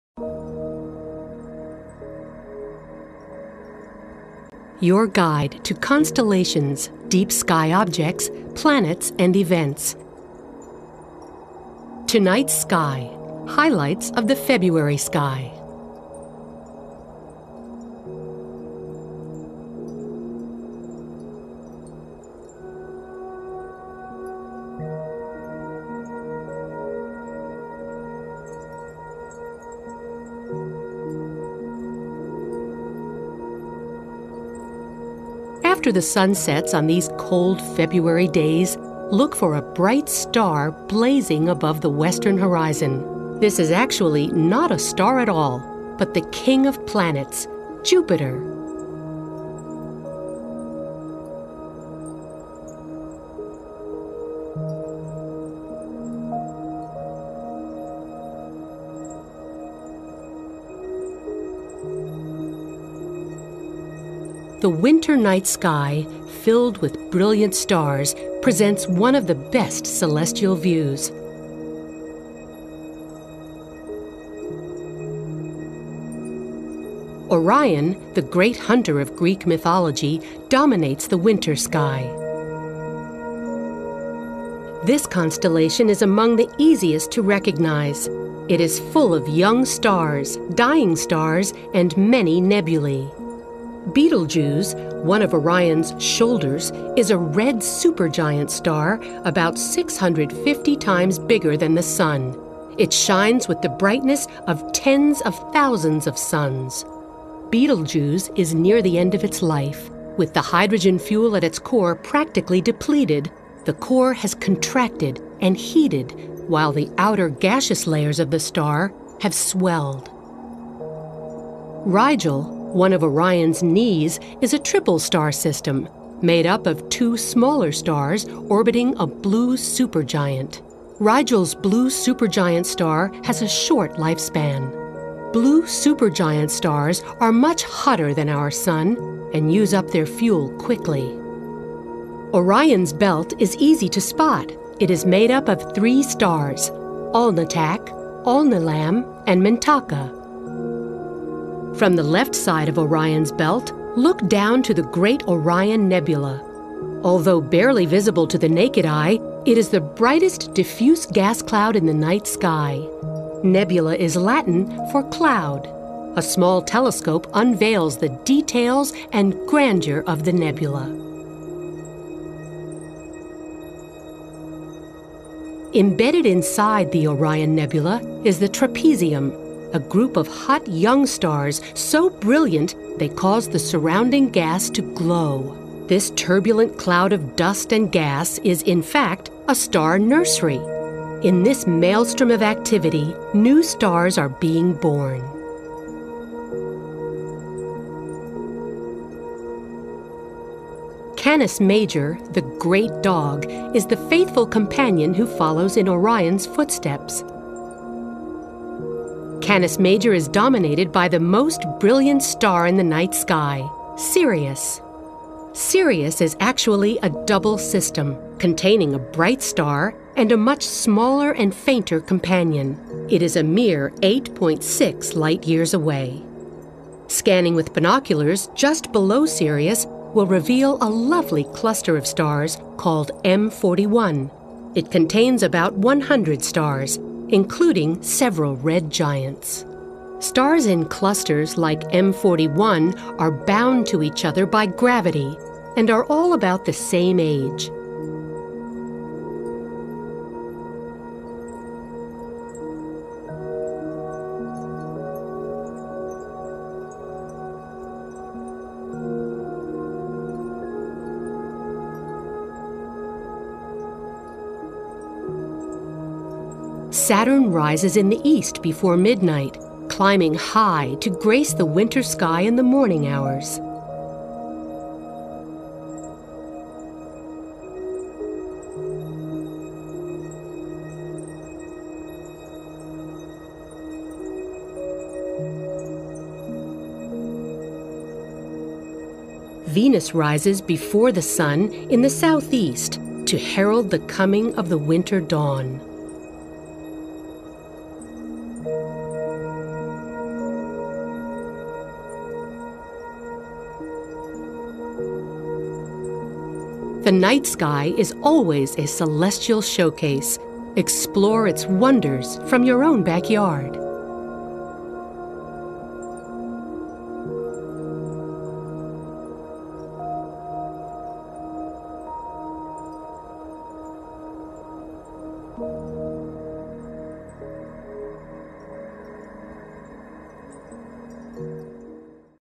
EL CIELO DE FEBRERO 2011 (HEMISFERIO NORTE)
Video realizado por el equipo del Hubblesite y traducido al castellano que muestra lo que podemos ver en el cielo el mes de Febrero de 2011 de manera amena y algo distinta